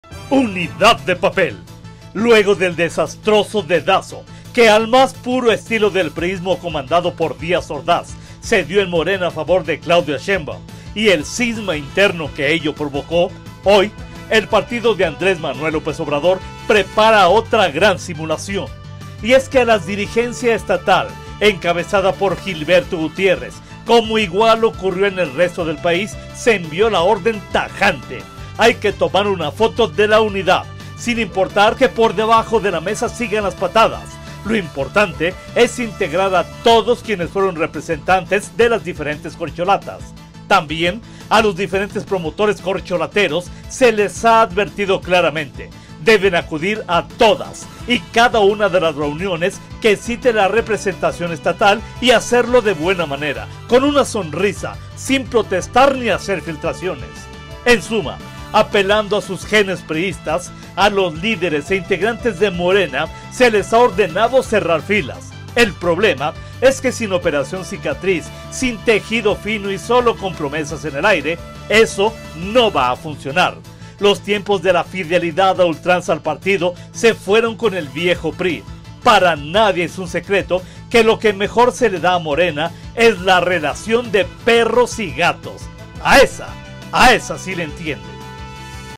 Crónica del Poder/Unidad de papel
Unidad de papel

Aguascalientes, Ags. 14 septiembre 2023.- Luego del destroso dedazo que, al más puro estilo del priismo comandado por Díaz Ordaz se dio en Morena a favor de Claudia Sheinbaum y el cisma interno que ello provocó, hoy el partido de Andrés Manuel López Obrador prepara otra gran simulación.
Y es que a las dirigencia estatal encabezada por Gilberto Gutiérrez, como igual ocurrió en el resto del país, se envió la orden: hay que tomar una foto de la “unidad”, sin importar que por debajo de la mesa sigan las patadas, lo importante es integrar a todos quienes fueron representantes de las diferente corcholatas.
También a los diferentes promotores corcholateros se les ha advertido: deben acudir a todas y cada una de las reuniones que cite la representación estatal y hacerlo de buena manera, con una sonrisa y sin protestar o hacer filtraciones.
En suma, apelando a sus genes priistas, a los líderes e integrantes de Morena se les ha ordenado cerrar filas. El problema es que, sin operación cicatriz, sin tejer fino y solo con promesas, eso no va a funcionar. Los tiempos de la fidelidad a ultranza al partido se fueron con el viejo PRI, para nadie es un secreto que lo que a Morena mejor se le da es la relación de perros y gatos, a ésa sí le entienden.